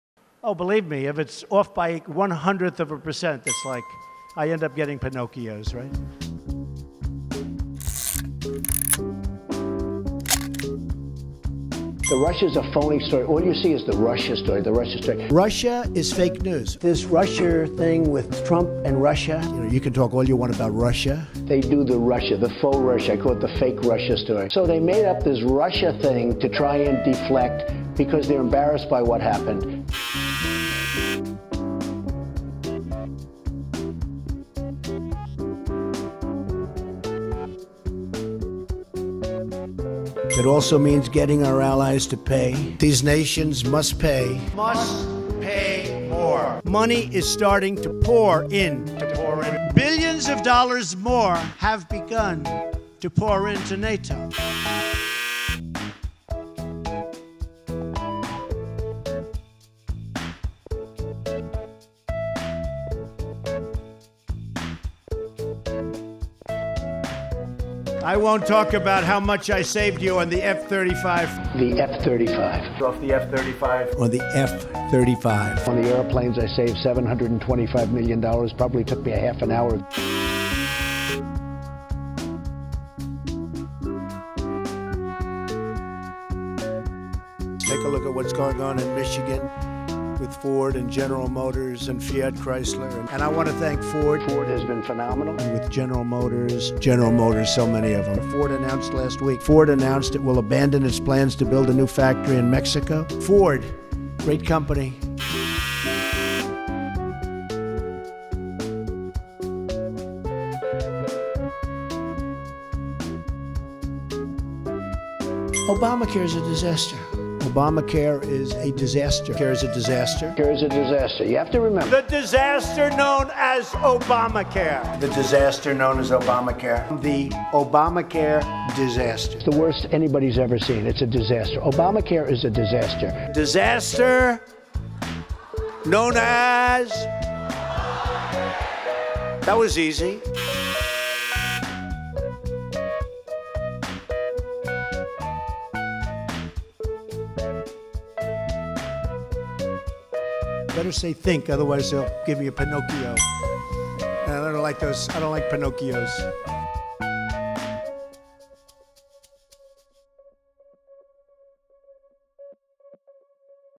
President Trump's top 5 misleading claims, so far
Six months into President Trump's term, he has made 836 misleading claims, averaging 4.6 per day.